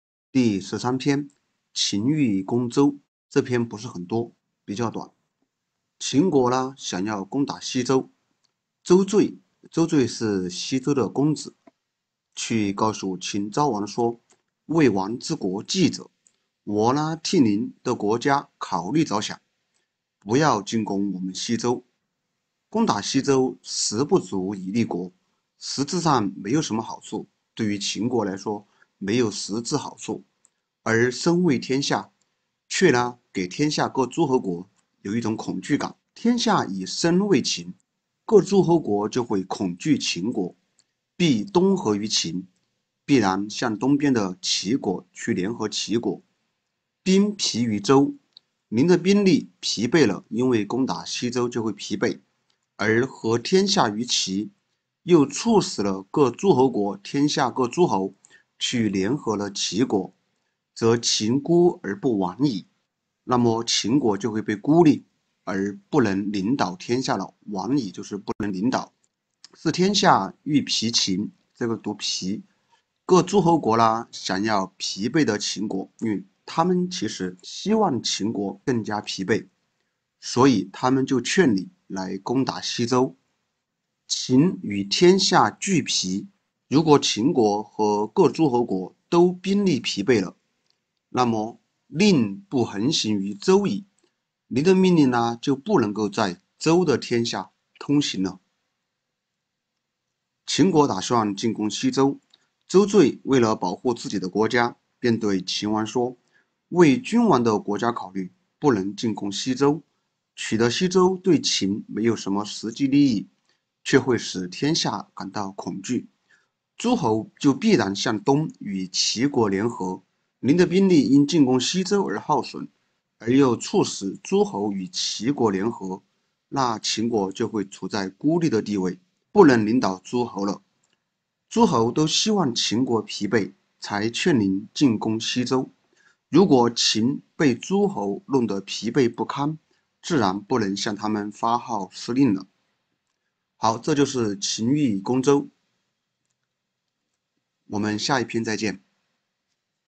《战国策》西周策《秦欲攻周》全文解读翻译 文白对照 文言文解释
秦欲攻周，周最谓秦王曰：“为王之国计者，不攻周。攻周，实不足以利国，而声畏天下。天下以声畏秦，必东合于齐。兵弊于周，而合天下于齐，则秦孤而不王矣。是天下欲罢秦，故劝王攻周。秦与天下惧罢，则令不横行于周矣。”